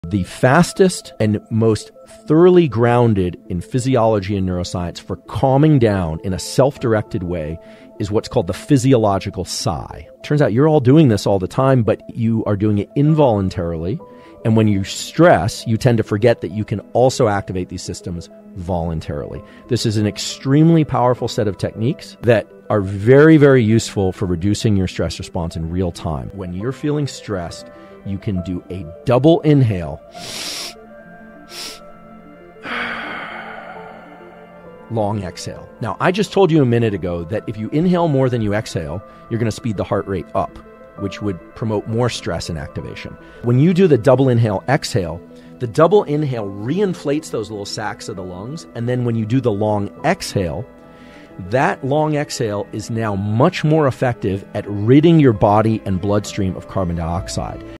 Neuroscientist: how to IMMEDIATELY calm DOWN
this is a very famous and powerful technique to calm down instantly as showed by andrew huberman, if you want to check out the full-length video here's the

Andrew D. Huberman is an American neuroscientist and tenured associate professor in the Department of Neurobiology and Psychiatry and Behavioral Sciences at Stanford University School of Medicine who has made contributions to the fields of brain development, brain plasticity, and neural regeneration and repair.